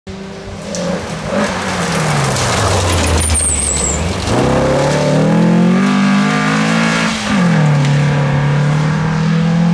Volkswagen Beetle Rally Car
Volkswagen Bug Rally Car - Old classic car used for rallying!
More videos on my channel:  @SwedishRally